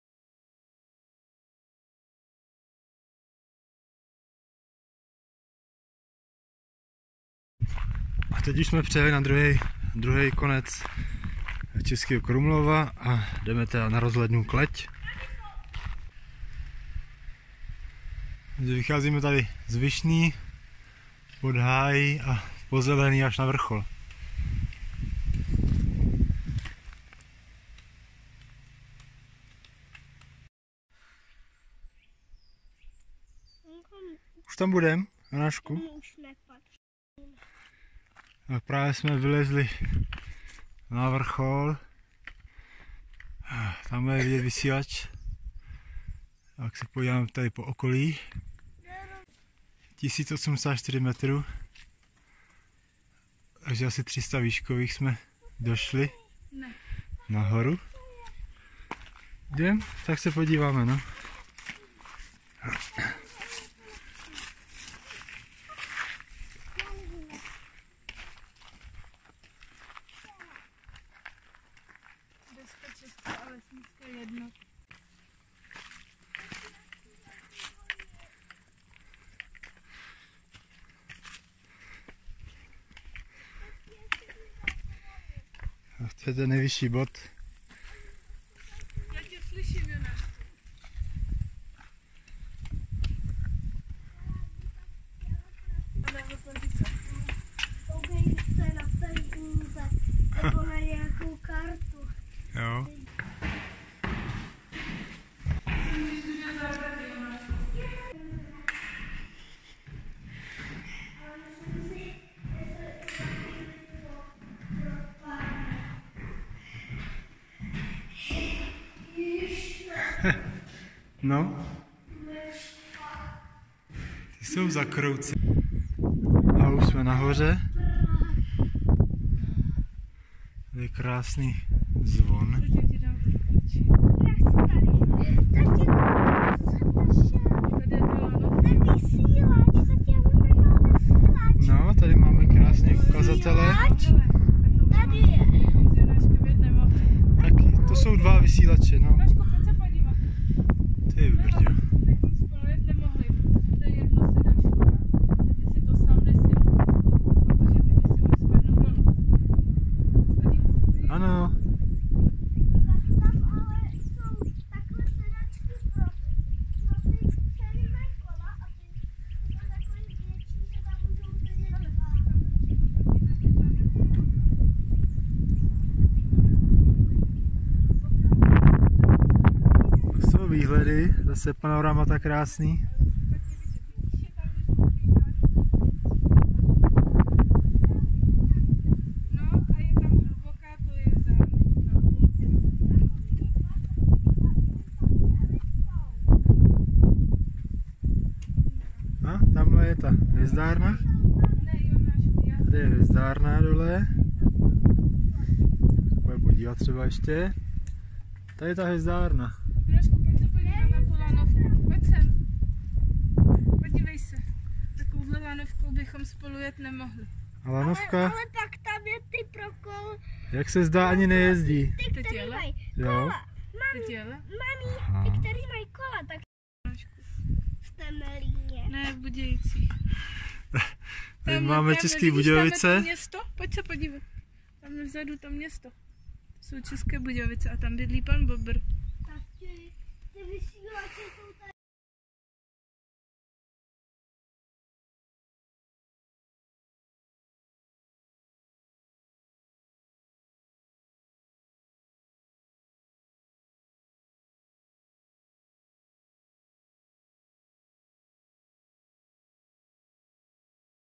Rozhledna Kleť krásné výhledy na jižní Čechy 7.10.2022
Kleť je s nadmořskou výškou 1087 m n. m. nejvyšší hora Blanského lesa, který je součástí Šumavského podhůří. Leží 6 km severozápadně od Českého Krumlova Na jejím vrcholu stojí nejstarší kamenná rozhledna v Čechách – Josefova věž, a roubená horská chata s restaurací chráněná jako kulturní památka – Tereziina chata.